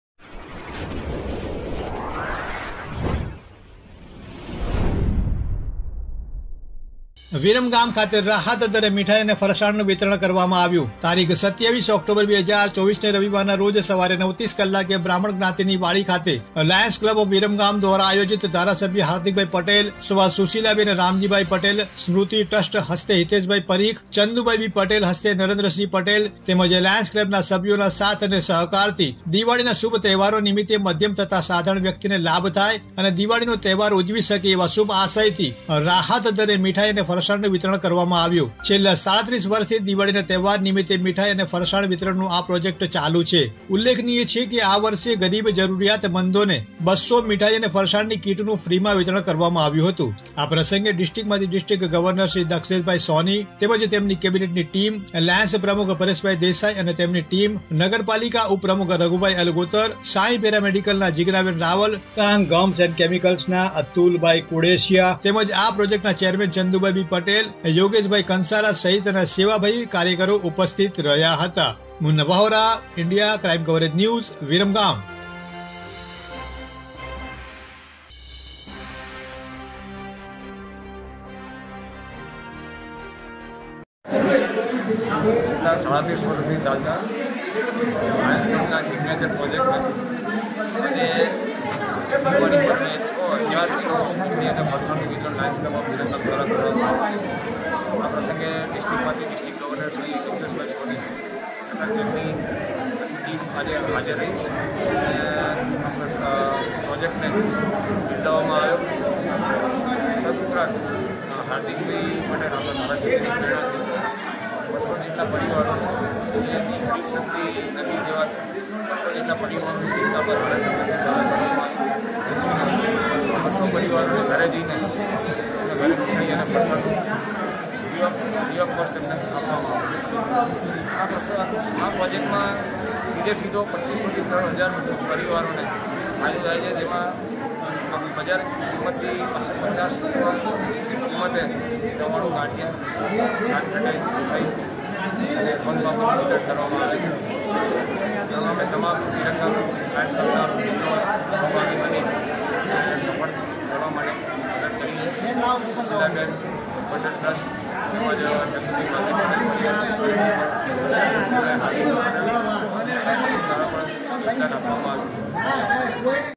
*વિરમગામ ખાતે રાહત દરે મીઠાઈ અને ફરસાણ નું વિતરણ કરવામાં આવ્યું*viramgam
*વિરમગામ ખાતે રાહત દરે મીઠાઈ અને ફરસાણ નું વિતરણ કરવામાં આવ્યું